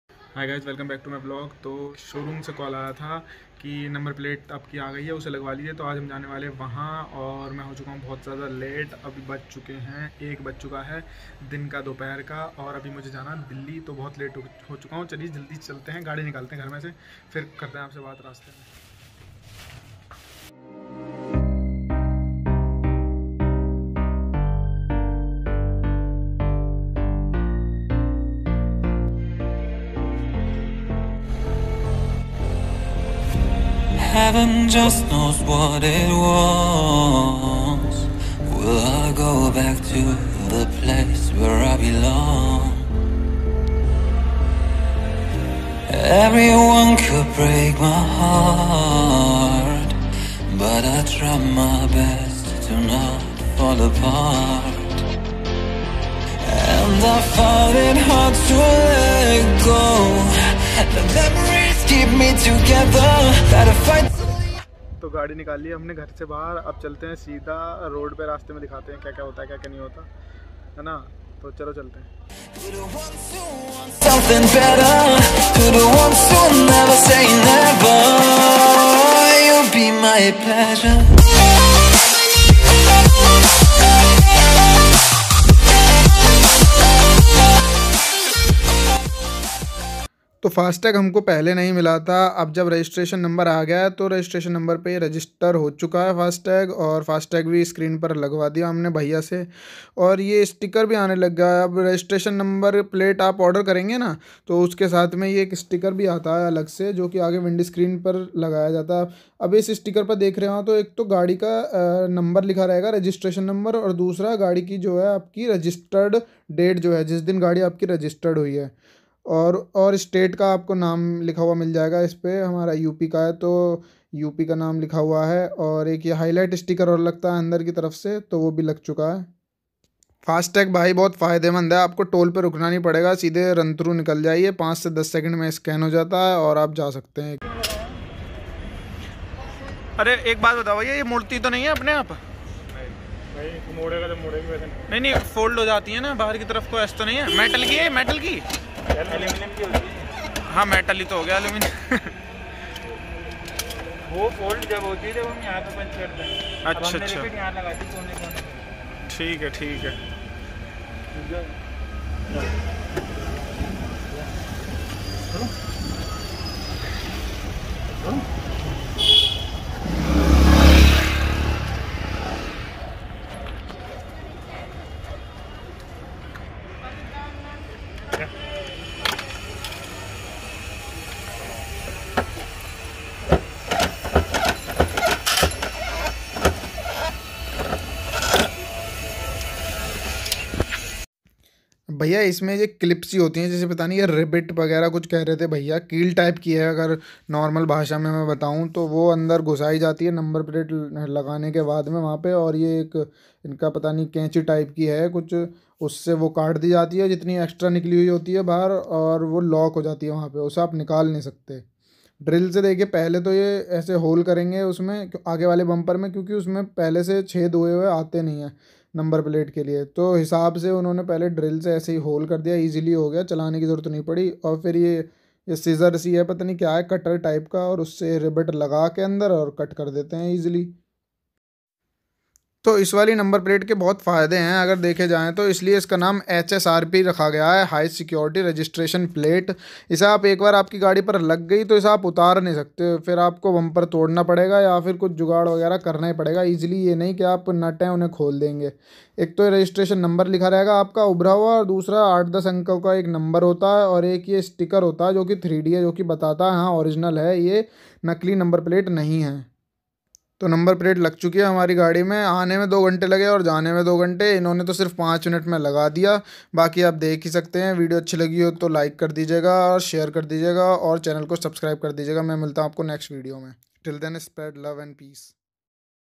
How HSRP installed🤔 | Ecosport Titanium S is now permanently registered😍 in RTO|PiyushK Vlogs 😎
my Ecosport Titanium S is now permanently registered in RTO and registration number mil chuka h,

HSRP ke kya fayeda hn or nuksaan
kitni secure h ye HSRP
ALL details covered
explained the HSRP
MY views on HSRP
process of installing the HSRP
how HSRP is installed